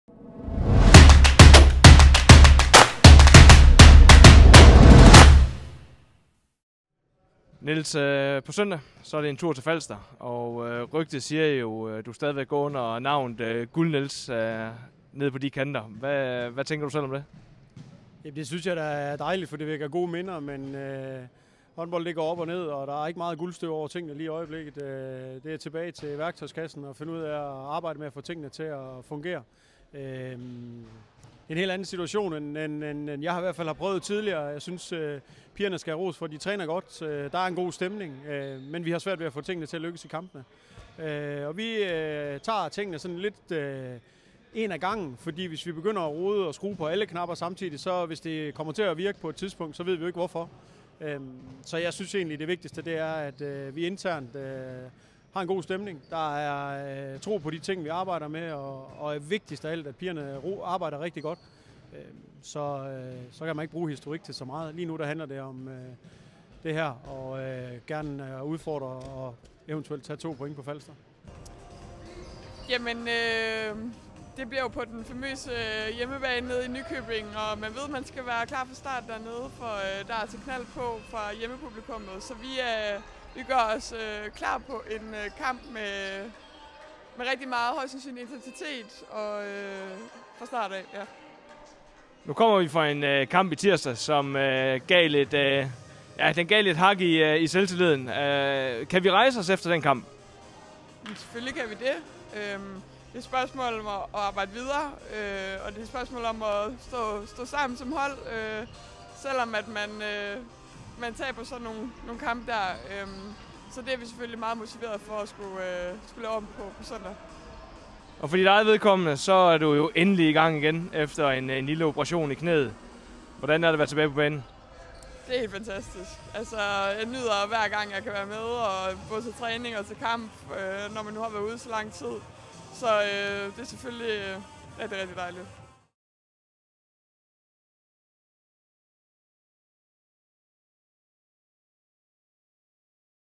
Optakt NFH - Randers HK
Vi tog en snak med Anne Sofie Hjort og Niels Agesen forud for kampen på Falster.